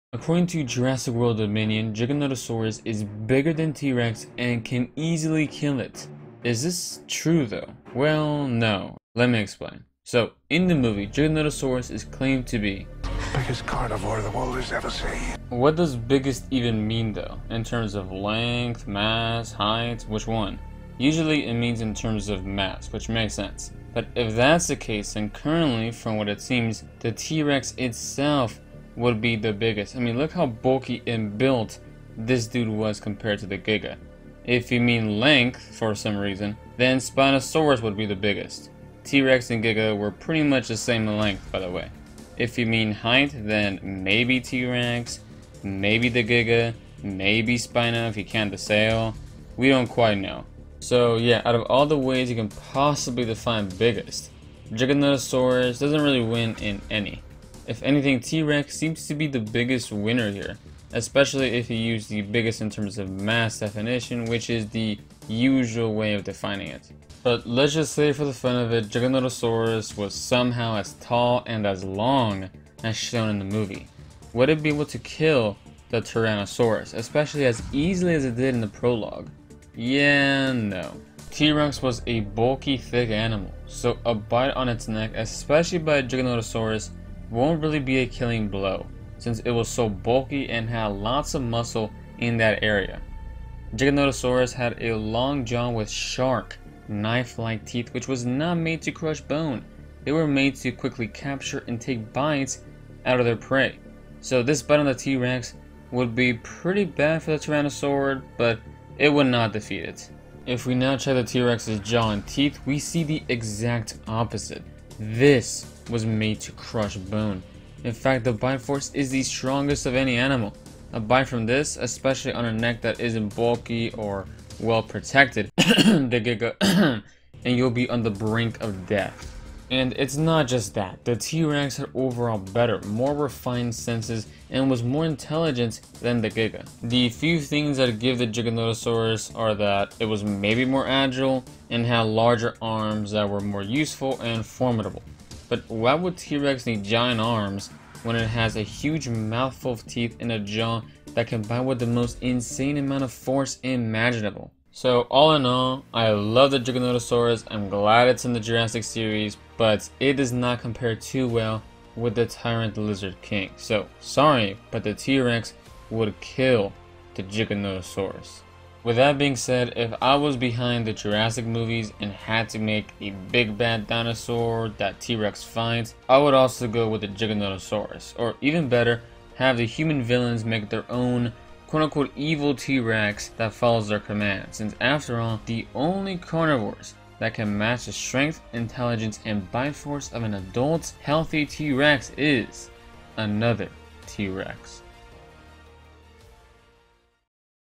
T. Rex Would Actually KILL Giganotosaurus (Sorry)
Jurassic World Dominion says Giganotosaurus can kill T. Rex; let's see why Jurassic World is scientifically WRONG about this and why T. Rex is actually the one who would win